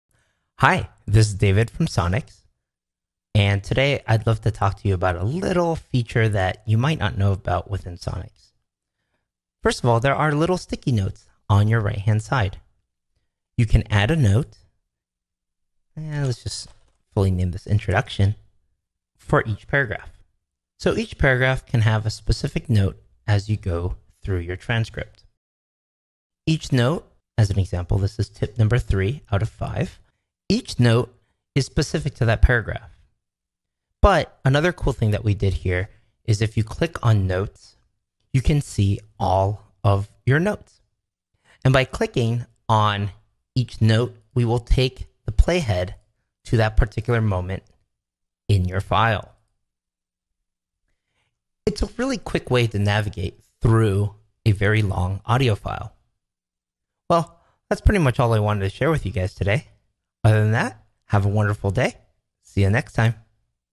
Sonix: How to use notes within the Sonix editor
We've built a quick way to navigate through your transcript with our notes feature. Quickly add a note to a paragraph within Sonix. Then, from the notes menu, you can jump quickly between these paragraphs.

It's a short and fun way to navigate through longer transcripts. Think of notes as a table of contents that just works magically.